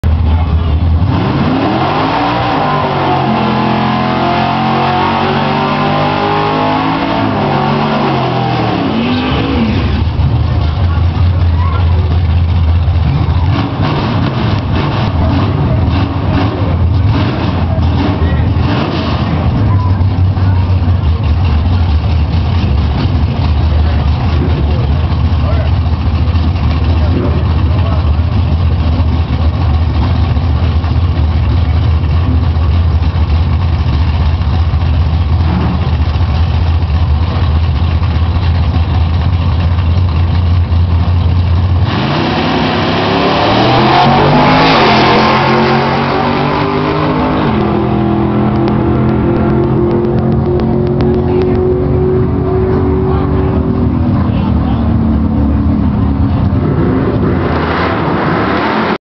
Bowtie's first run of 08 with new 406 combo.
7.40 pass @ 92 mph letting out just past 1/8 mile. Car has currently made a best pass of 7.17 @ 96mph 1/8 mile. Current best 1/4 mile is 11.40 @ 118mph.